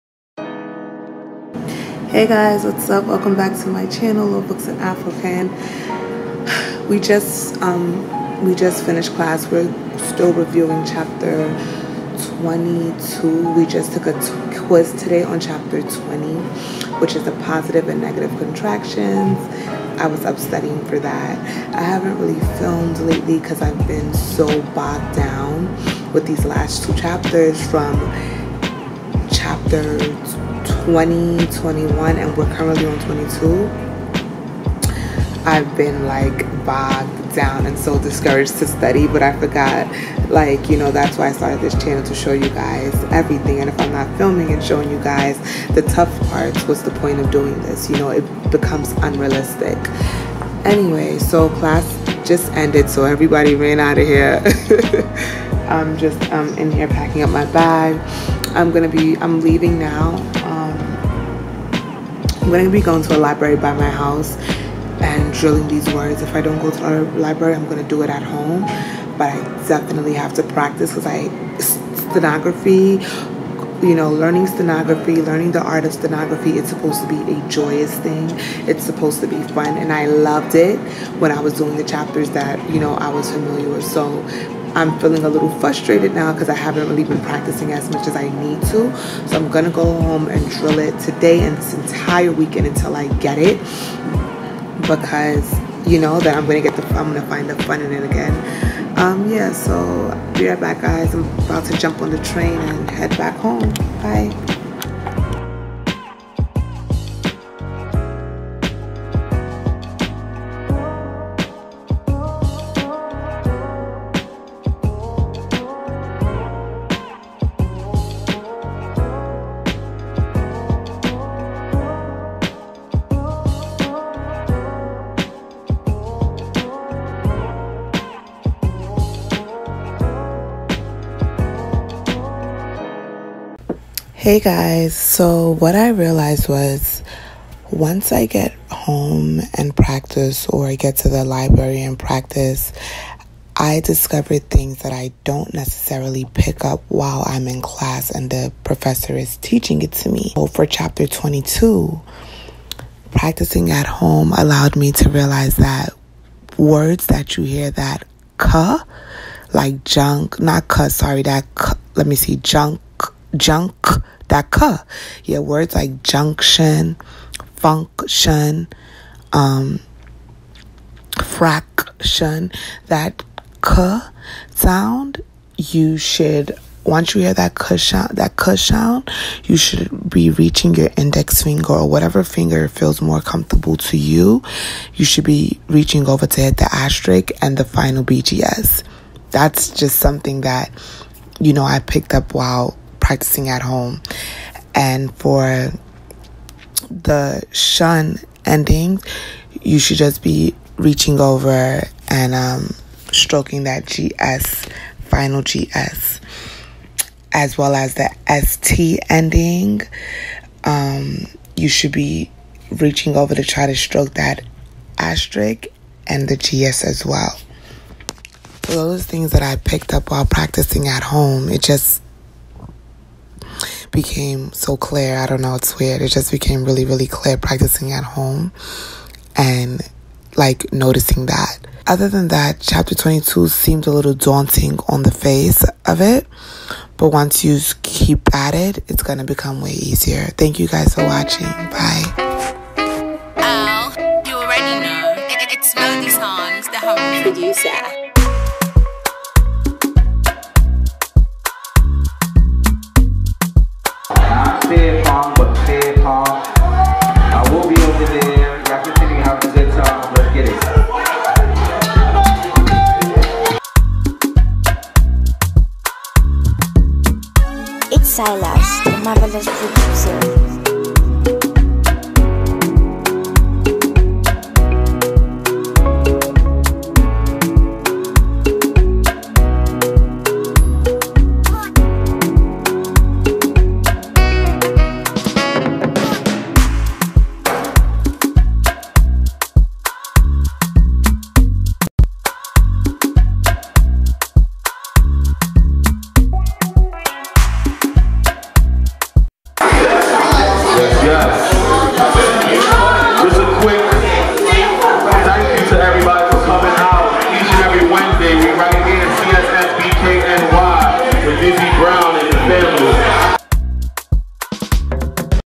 Court Reporting School Takes Practice | Tackling Chapter 22 + Game Night| LegallyEmoyTV
Court Reporting School Takes Practice. Also lots of dedication. Learning chap. 22 felt like a nightmare that wouldn’t go away until. I put my fear aside and tackled it head TP EU*-RS😜